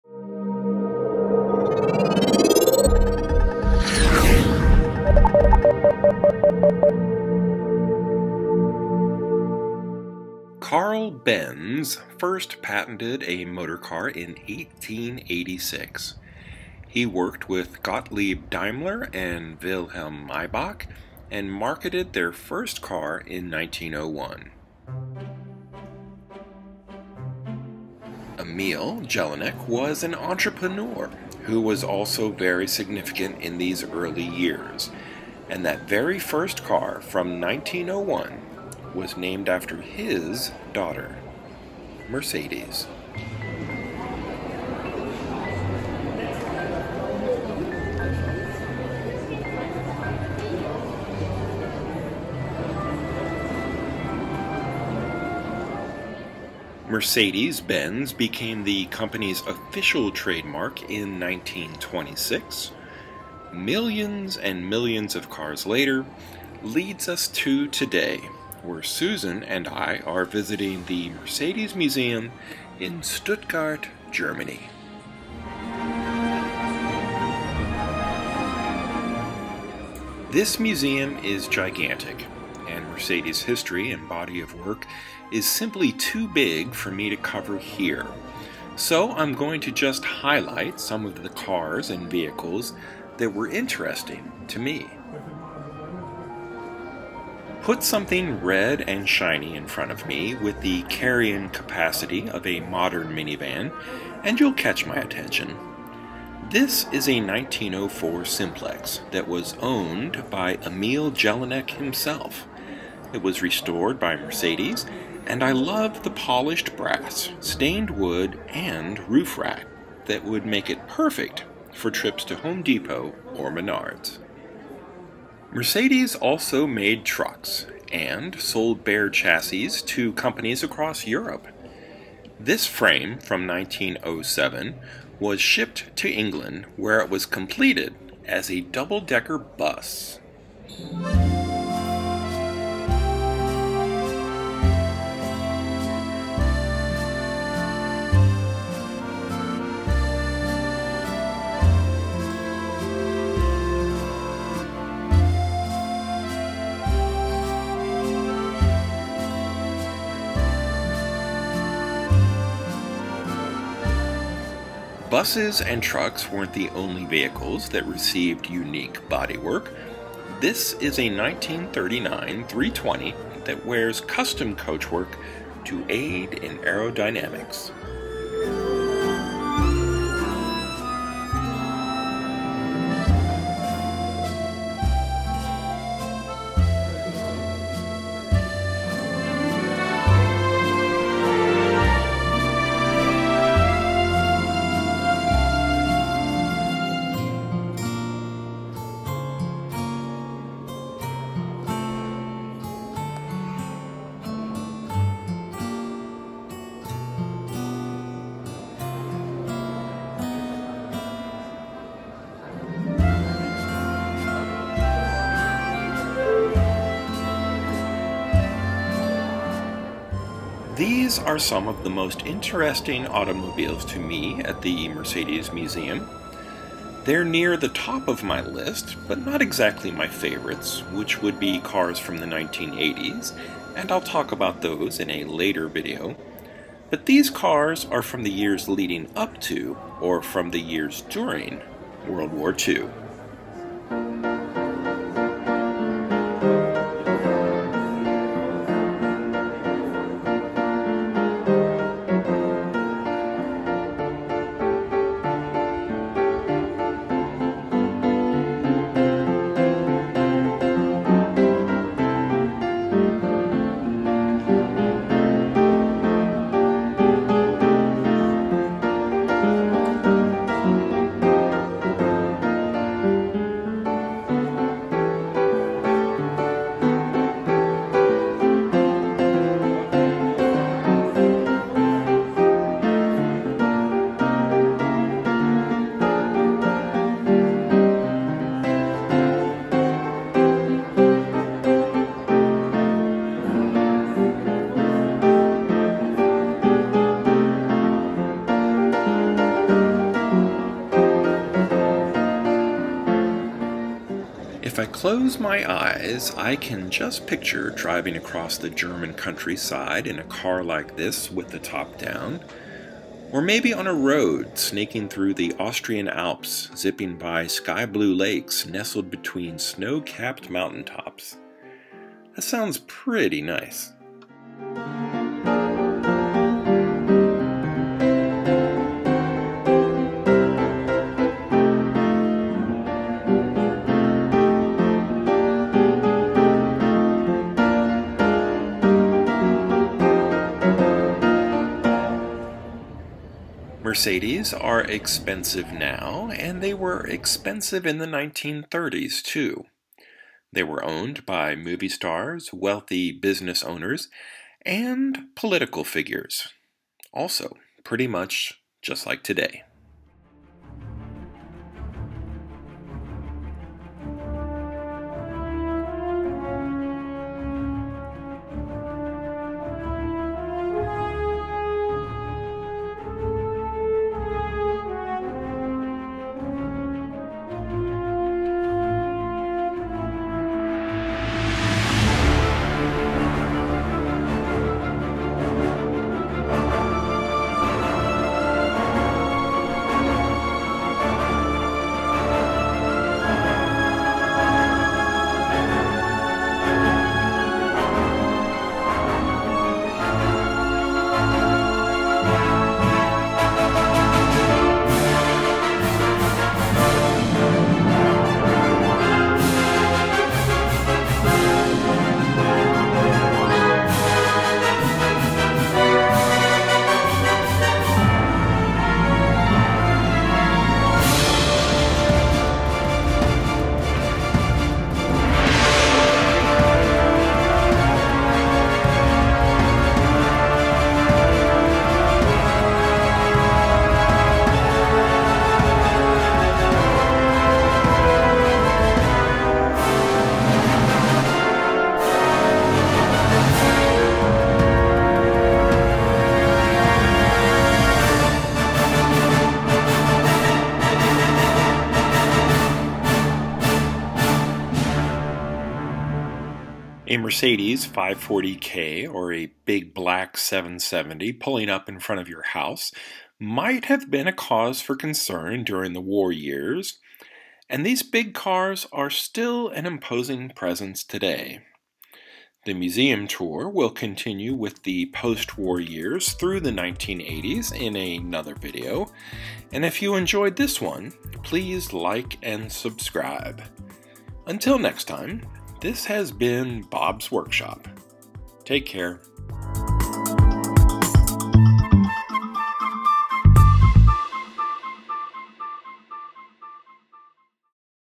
The Mercedes Museum in Stuttgart Germany 2023
We recently went to Germany and Austria and were lucky enough to check out the Mercedes Benz Museum in Stuttgart. It's an incredible display of the automobiles, trucks, buses and other vehicles that Mercedes has made since the 19th Century. Yes, that's right. Karl Benz first made a motorized vehicle in 1886. This video hits some of the highlights from that beginning through the Second World War. There will be more videos to follow.